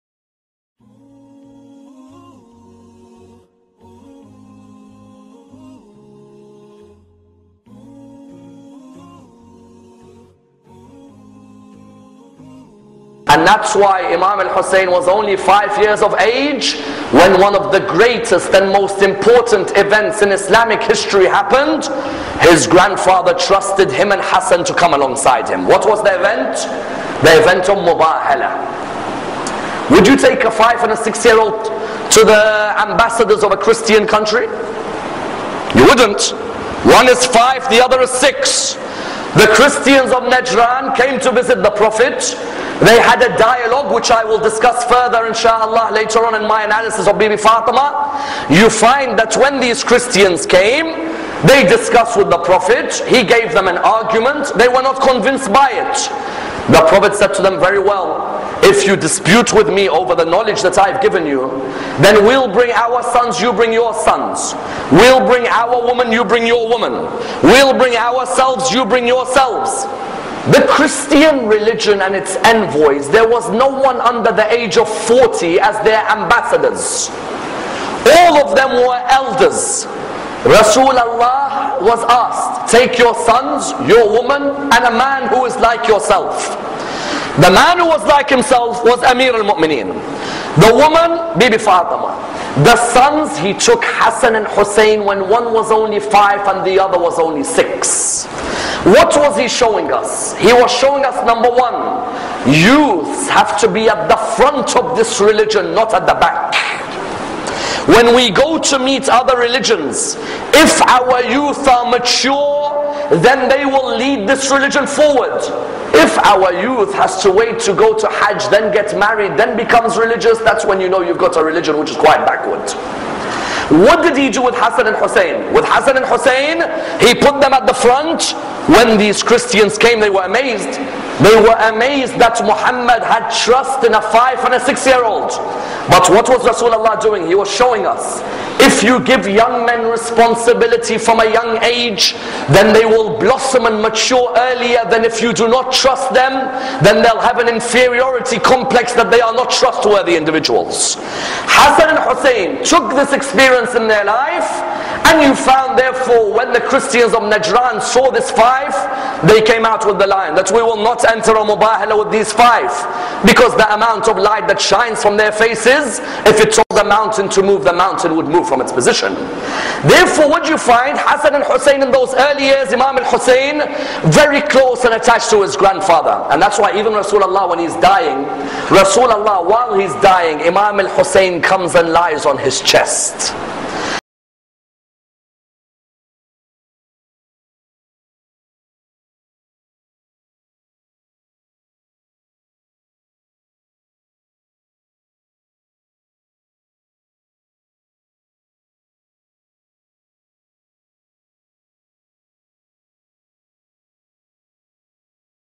Important Lesson For Parents and Youth on Eid Mubahila - Sayed Ammar Nakshawani
The Event of Mubahila Has an Important message for the youth as well as their parents. When Prophet Muhammad takes a 5-year-old against the Christians of Najran, what message was he giving to the community in the 21st Century?

Ammar Nakshawani highlights a well though point.